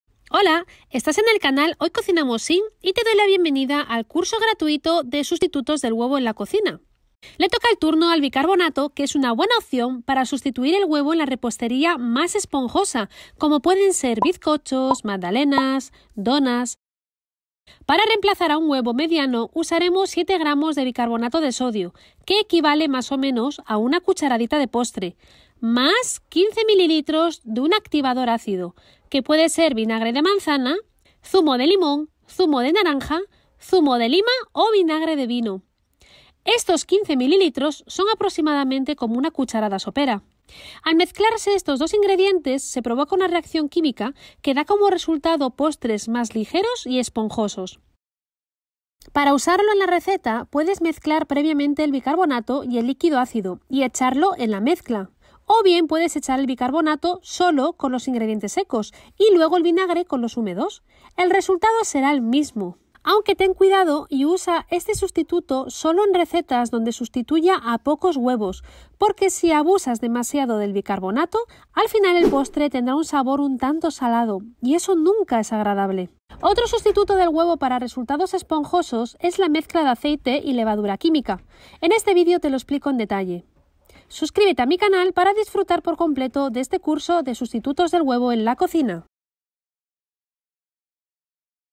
🥚Sustituir el huevo por BICARBONATO para Repostería Esponjosa 🥐 (Vídeo 1 - Curso Gratuito)
¡Te doy la bienvenbida al CURSO GRATUITO Online de SUSTITUTOS DEL HUEVO en la Cocina! Una serie de vídeos donde EXPLICO uno por uno TODOS los Sustitutos posibles del huevo y el uso que le puedes dar para preparar postres sin huevo.

En este vídeo nos centramos en el bicarbonato que es una buena opción para sustituir el huevo en la repostería más esponjosa como pueden ser queques, bizcochos, tortas, cupcakes, donas, donuts, magdalenas.

¿Cómo reemplazamos un huevo mediano por bicarbonato? Usaremos 7 gr de bicarbonato de sodio (que equivale más o menos a 1 cdta de postre) y le añadimos 15 ml de un ACTIVADOR ÁCIDO (que puede ser vinagre de manzana, zumo de limón, zumo de naranja, zumo de lima ó vinagre de vino) - Estos 15 ml son aproximadamente como 1 cucharada sopera.

Cuando mezclamos estos dos ingredientes que reemplazan al huevo se provoca una reacción química que da como resultado postres más esponjosos y ligeros, y todo sin usar huevo en la receta.

Este sustituto del huevo puedes usarlo de dos formas:
➖La primera preparas previamente la mezcla del bicarbonato y el activador ácido, y luego lo añades a la receta directamente.
➖La segunda forma es, que mientras estás preparando la receta, echas sobre los ingredientes secos el bicarbonato, y luego, cuando vayas a incorporar los ingredientes húmedos, añades el activador ácido (vinagre, limón...)

¡Cuidado! Usa este sustituto del huevo solo en recetas donde haya que sustituir a pocos huevos (Un huevo o dos como mucho) porque si abusas del bicarbonato el resultado será un postre salado... ¡Qué desagradable!

👇NAVEGA POR LAS PARTES PRINCIPALES DEL VÍDEO👇
0:00 -Curso Gratis Sustitutos del huevo en la cocina
0:07 -Cómo sustituir el huevo por Bicarbonato
0:17 -Reemplazar un huevo por sustituto vegano
0:35 -Sustituir huevo por bicarbonato y vinagre
0:53 -Sustituir huevo por bicarbonato y limón
1:15 -Cómo sustituir el huevo por aceite y levadura química